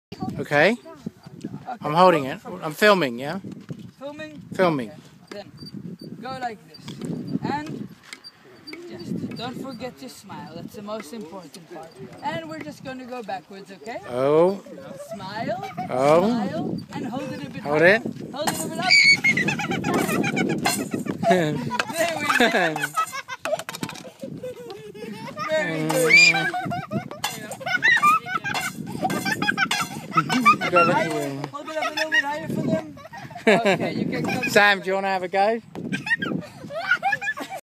Ostriches attack little girl
Little girl from UK feeds ostriches in Majorca and gets more than she bargained for!